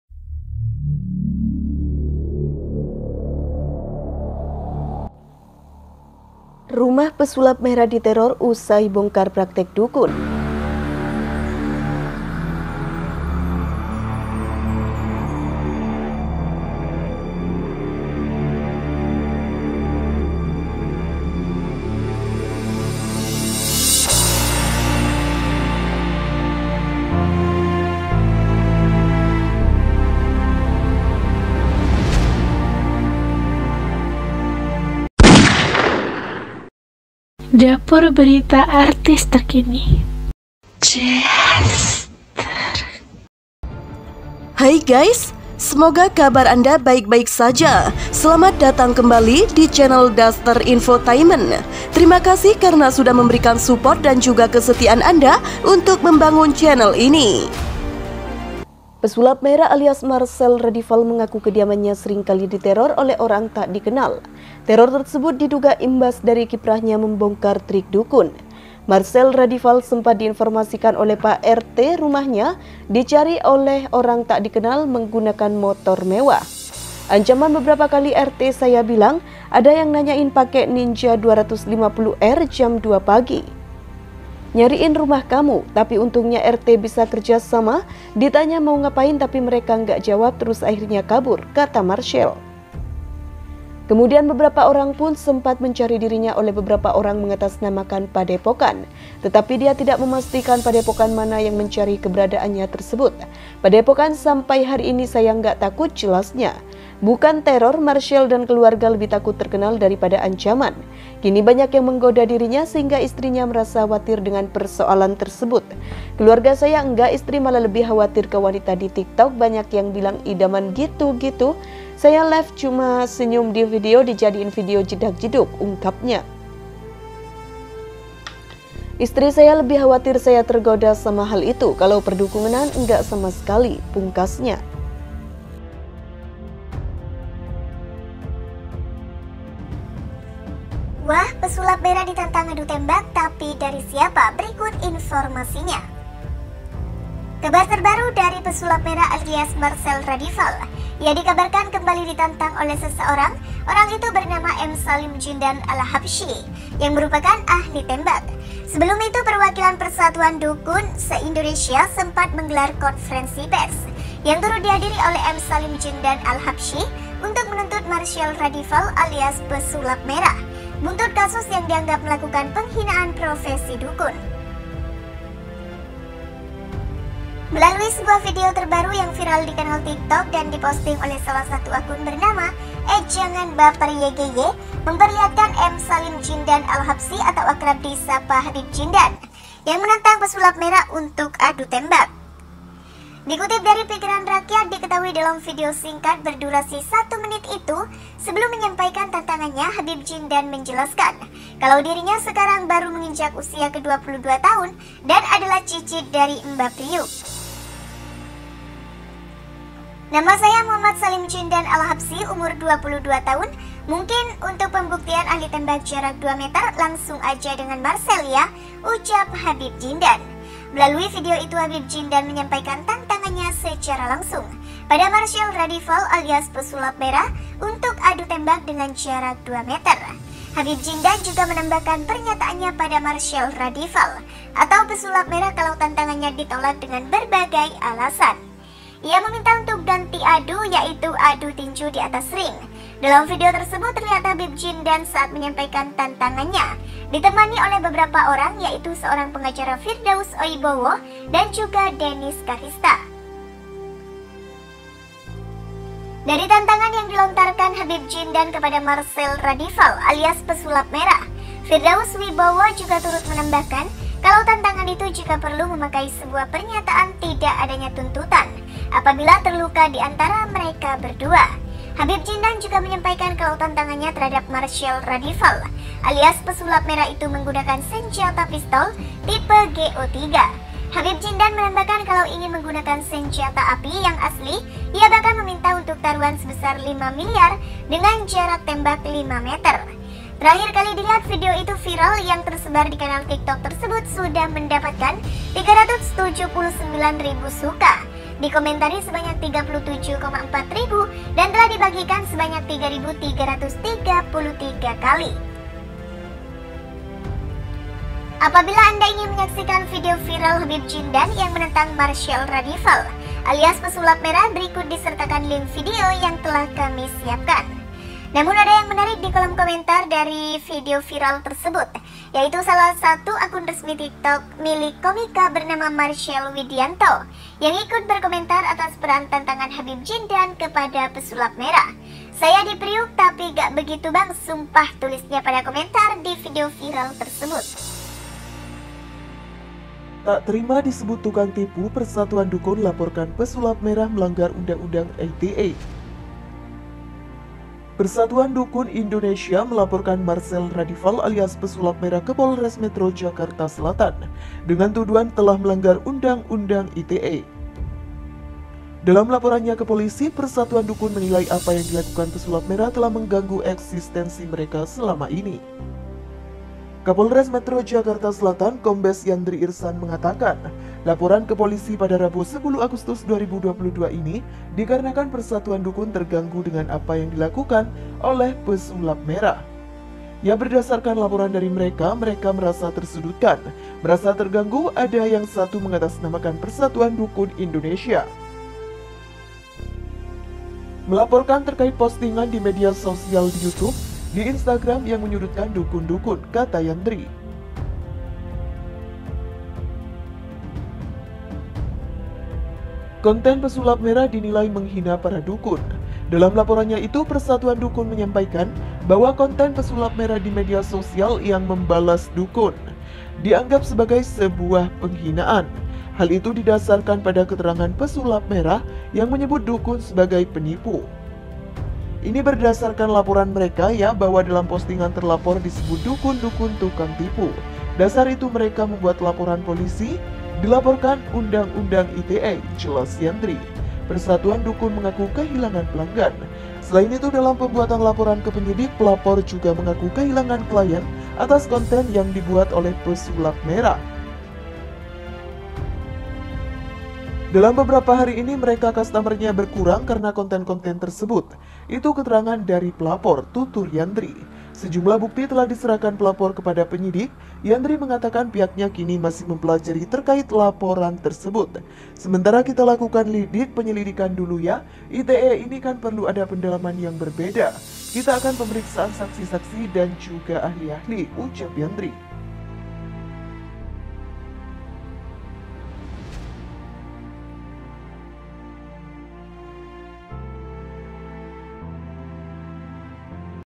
BONGKAR PRAKTIK DUKUN, RUMAH PESULAP MERAH DITEROR | gosip artis hari ini
insertlive,
insert today,
insert pagi,
insert siang,
infotainment,
indonesia,